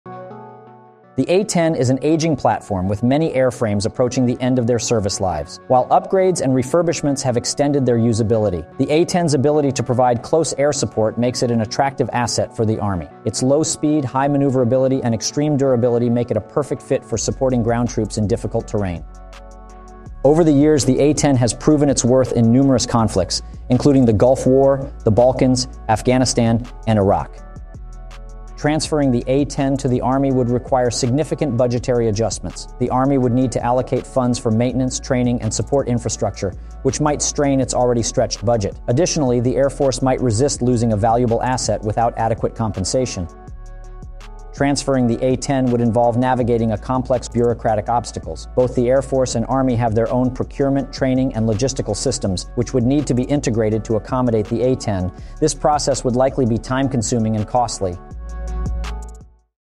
This is Why the A-10 aren’t flown by the Army
Transferring the A-10 Thunderbolt II from the Air Force to the Army is not a straightforward proposition. Budgetary constraints, bureaucratic challenges, and technological factors all contribute to the complexity of this issue. Instead, focusing on upgrading the existing A-10 fleet or developing a new CAS platform may be more practical and beneficial solutions for both services.

💯💯ACROSS MEDIA PRODUCT
Across Media is Independent channel. All profits from this shirt go to the team and content improvements.